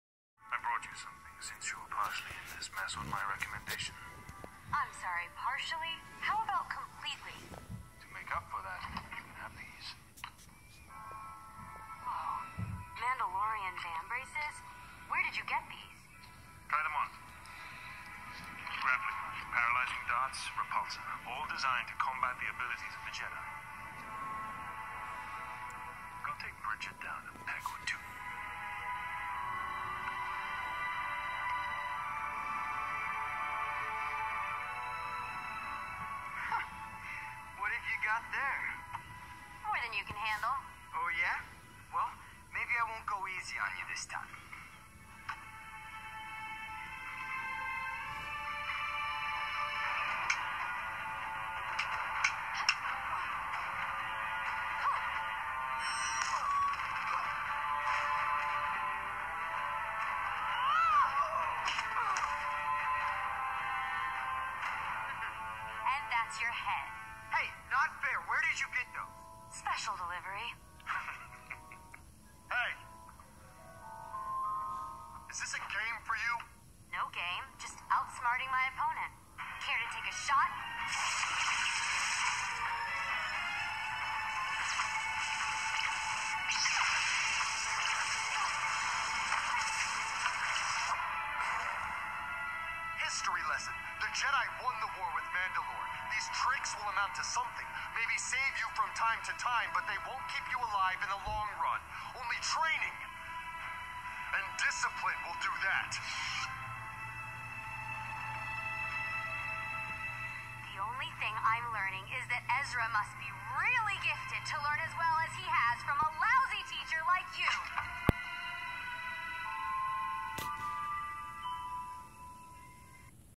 Second trailer of Trials of the darksaber
Sabine Goetz Mandalorian vambraces and battles Ezra